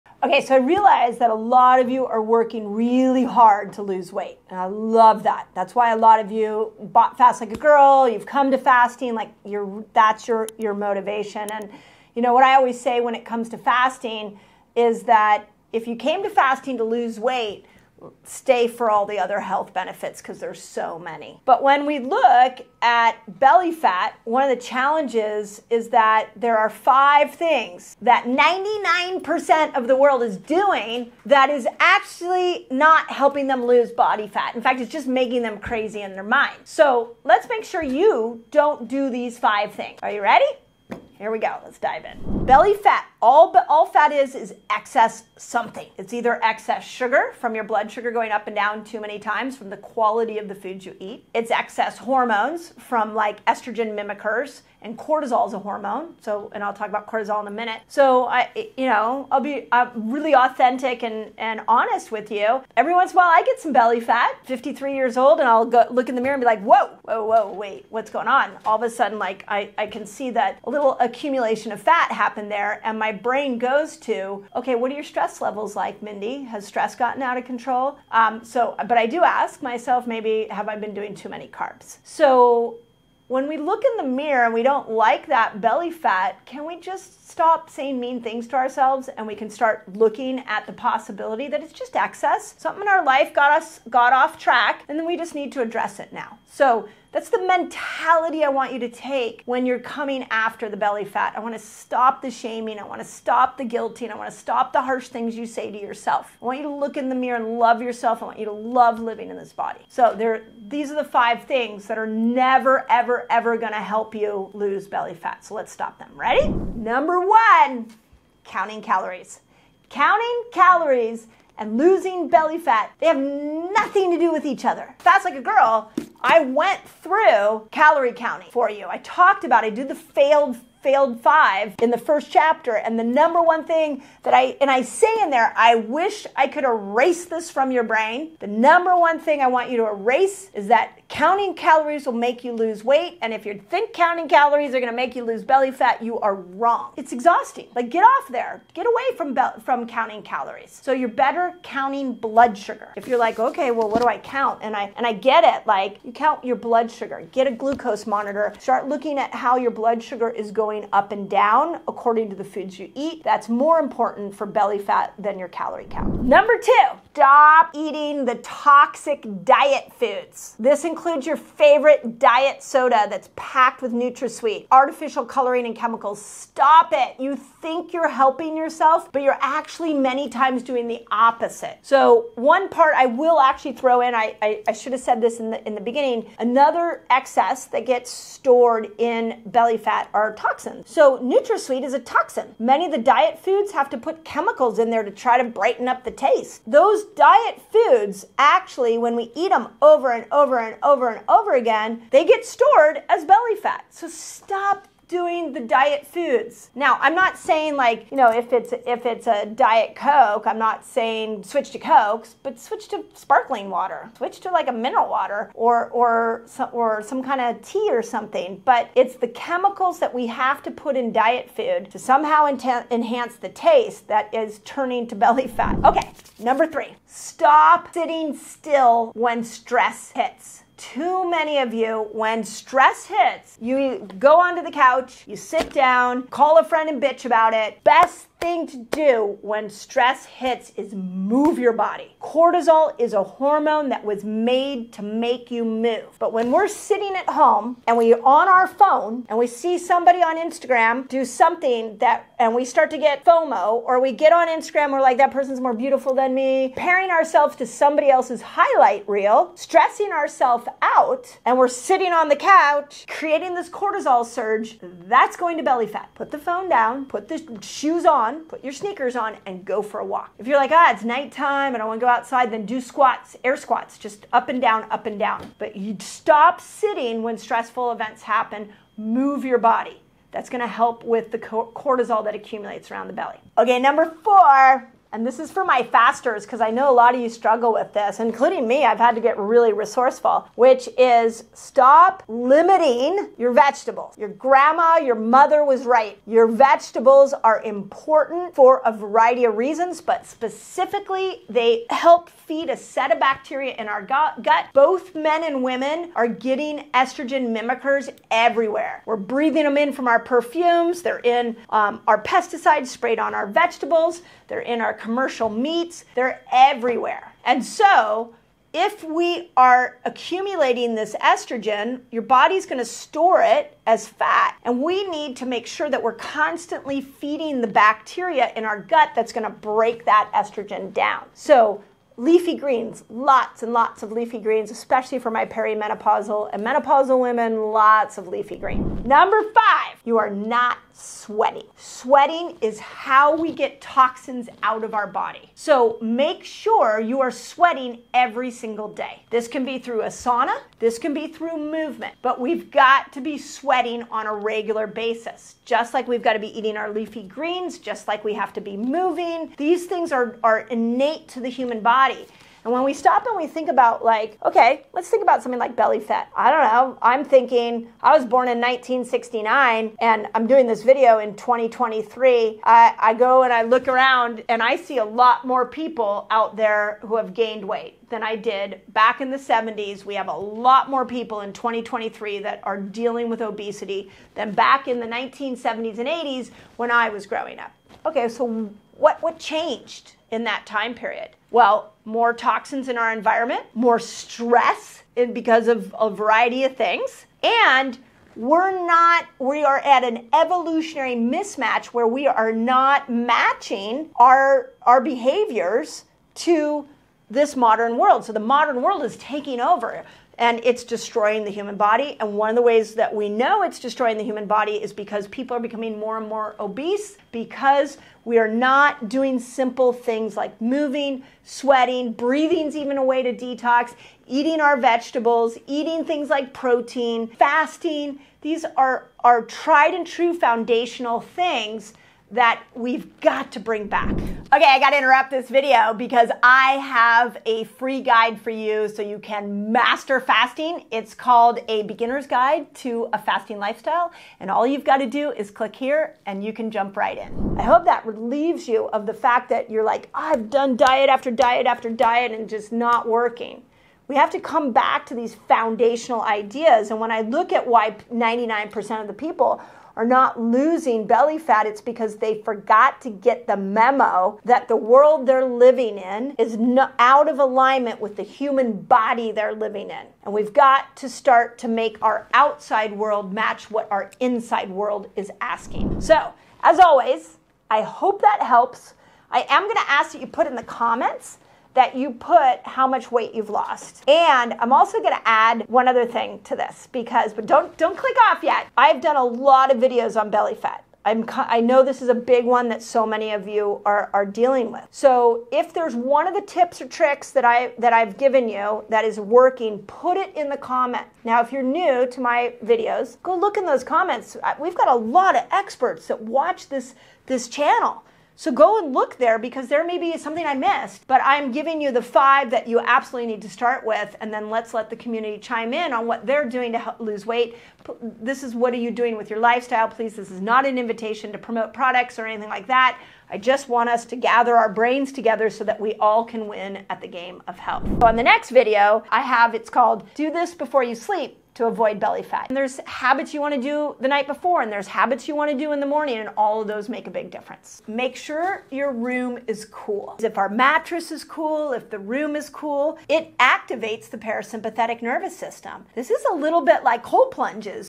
The 5 Reasons 99% Of People NEVER LOSE Belly Fat | Dr. Mindy Pelz
Sign up for the Beginner's Guide to a Fasting Lifestyle course
OPEN ME 👇 FOR RESOURCES MENTIONED